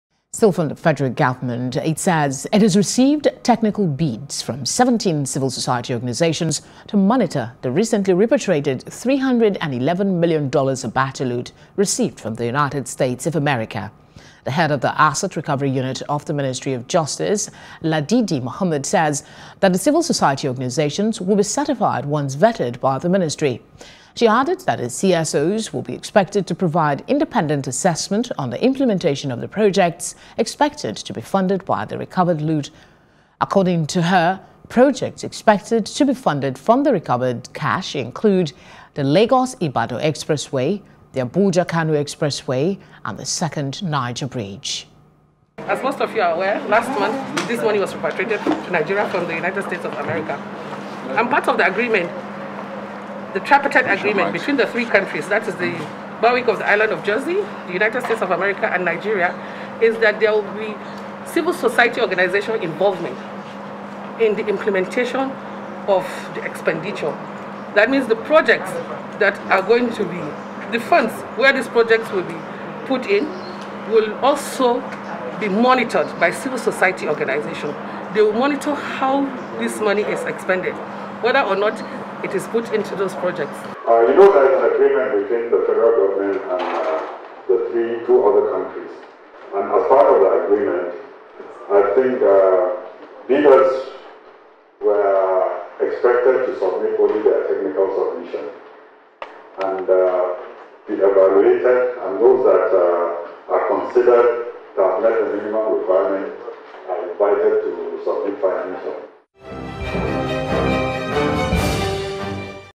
Abacha Loot: FG Commences Vetting Of Applications For Independent Monitors
The Federal government says it has received technical bids from seventeen civil society organizations to monitor the recently repatriated three hundred and eleven million dollars Abacha loot received from the United States of America.